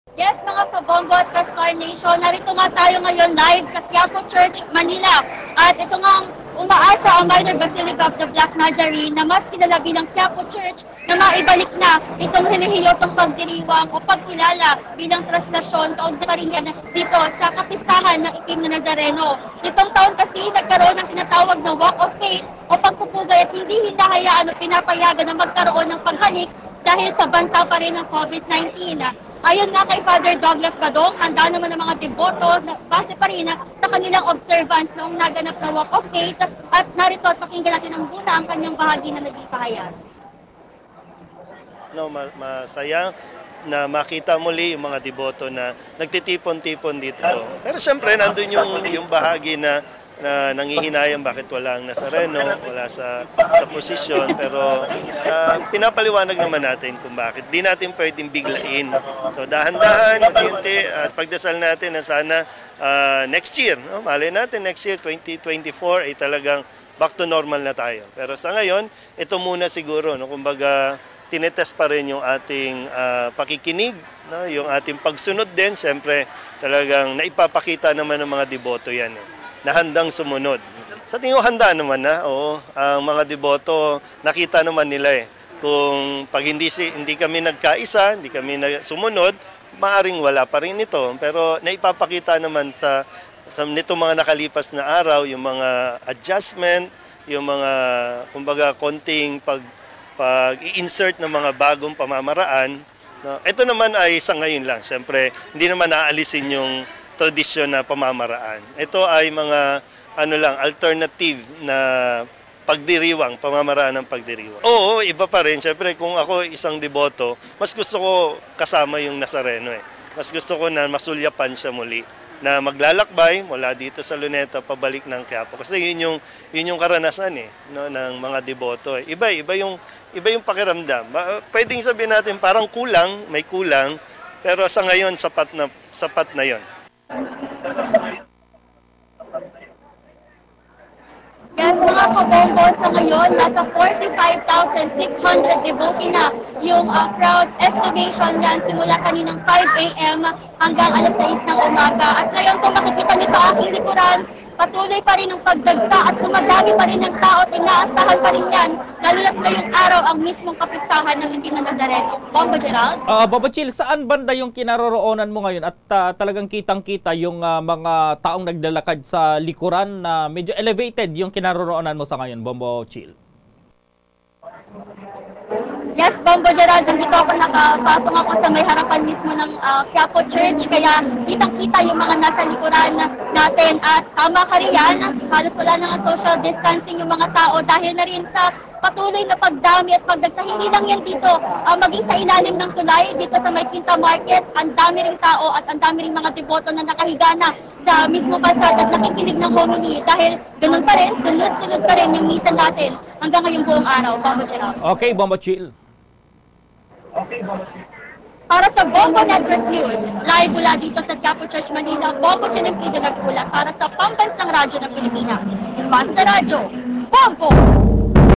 Simbahan ng Quiapo, umaasang magbabalik na ang tradisyunal na Trasclacion sa susunod na taon
Kasunod ng matagumpay na Walk of Faith, simbahan ng Quiapo, umaasang magbabalik na ang tradisyunal na Trasclacion sa susunod na taon